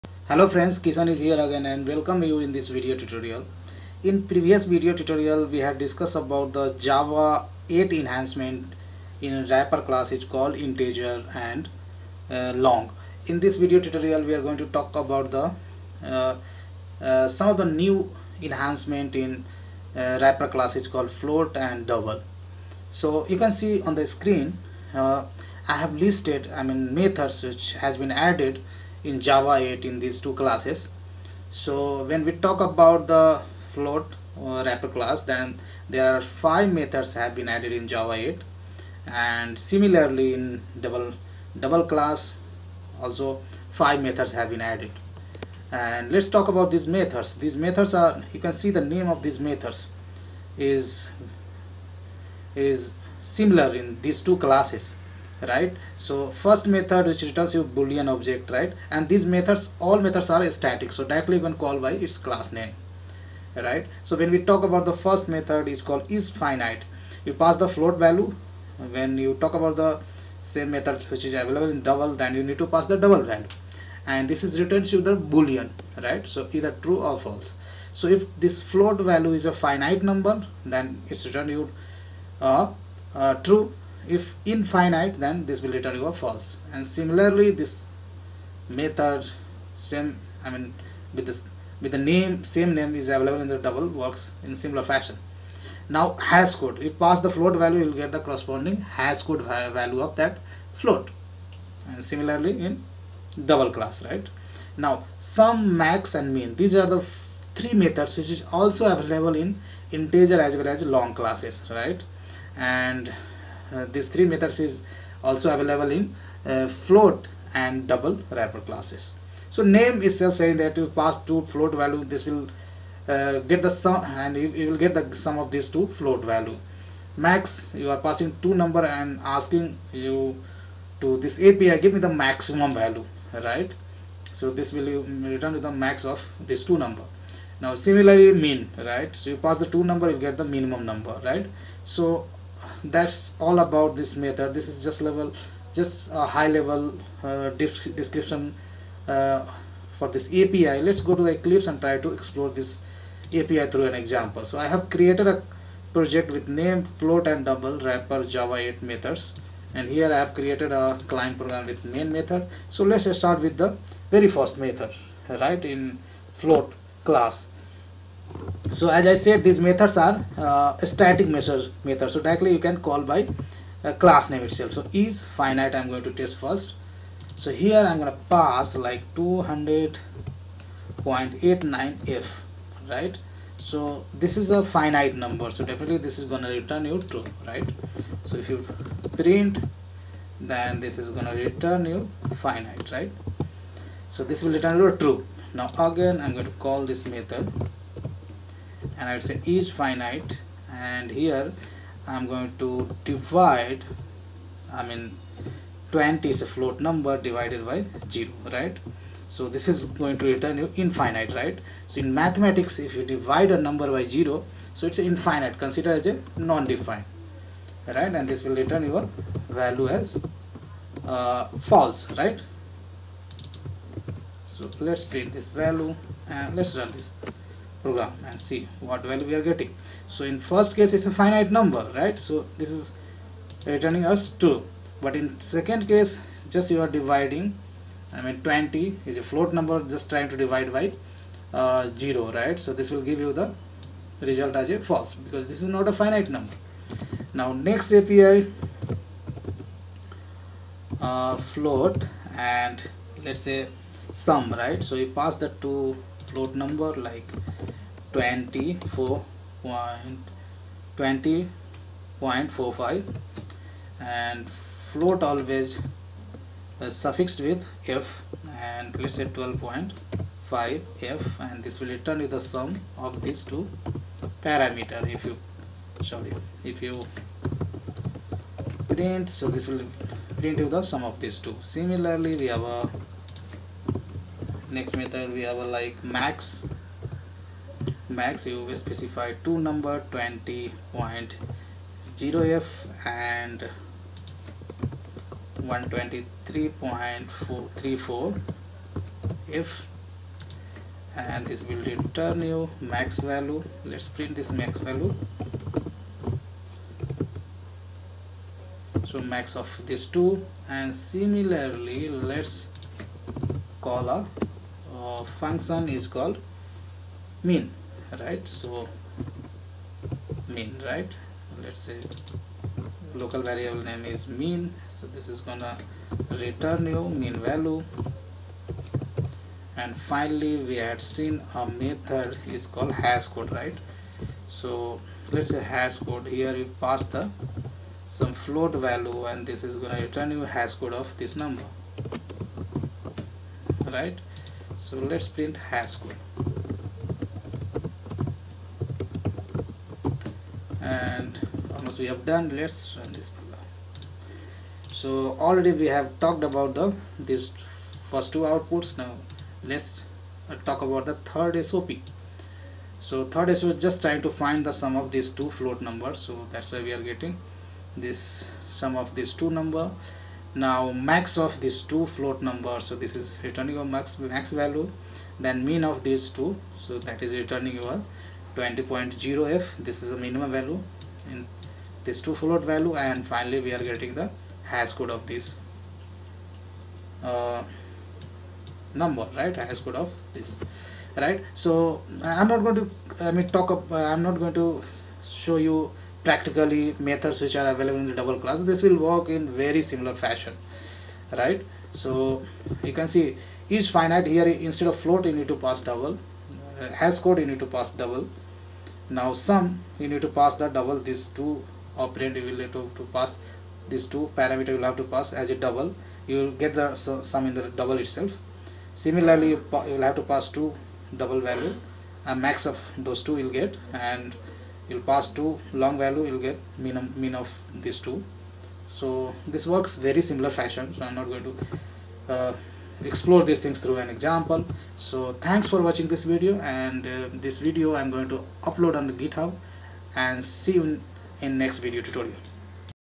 Java 8 Float & Double classes enhancements
In this video tutorial I will explain you how to work Java-8
Float & Double classes enhancements using a demo project
Below is the GitHub link to download source code: